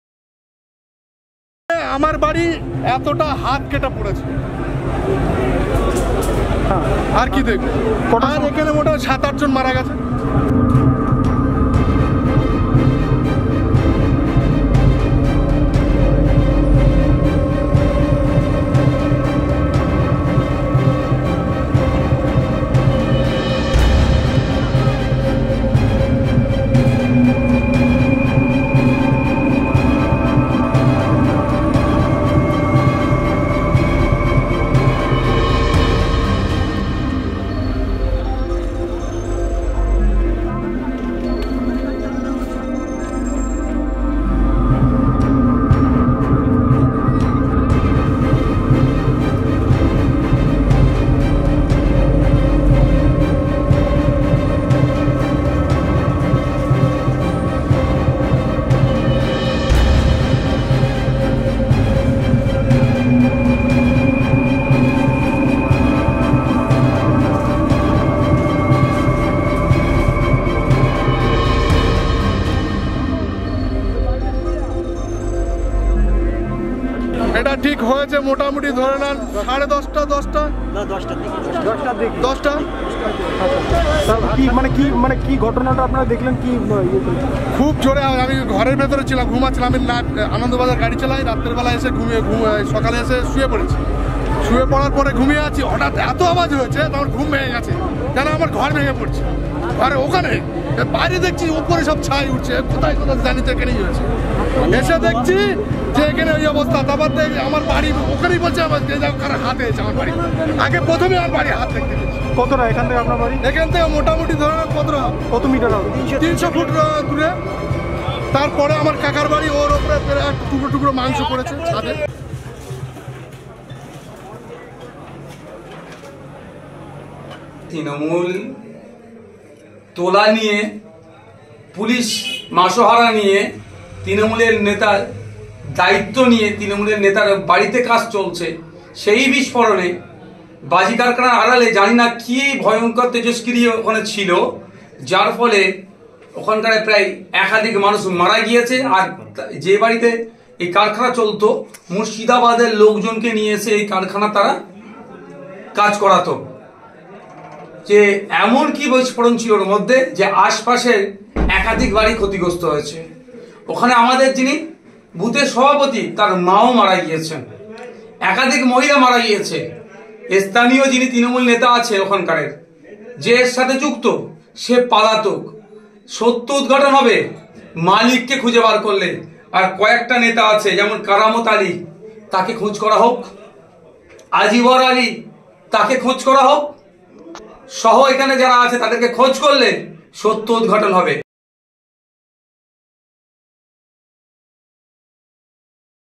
Duttapukur Blast: উড়ে গেল বাড়ির ছাদ, ছিন্ন ভিন্ন দেহ এসে পড়ল চারপাশে; বিভীষিকা দত্তপুকুরে | ieBangla
রবিবার সকালে বিকট শব্দে কেঁপে উঠল উত্তর ২৪ পরগনার দত্তপুকুর। বেআইনি বাজি কারখানায় ভয়াবহ বিস্ফোরণে মৃত্যু-মিছিল। প্রশাসনের নাকের ডগায় দিনের পর দিন ধরে বেআইনি বাজি তৈরি রমরমা কারবার চলছিল বলে অভিযোগ স্থানীয়দের। পুলিশ জেনেও আগে কোনও পদক্ষেপ করেনি বলে অভিযোগ। বিস্ফোরণের জেরে আশেপাশের বেশ কয়েকটি বাড়ি ক্ষতিগ্রস্ত। মারাত্মক বিস্ফোরণে ছিন্নভিন্ন হয়ে গিয়েছে বেশ কয়েকটি দেহ।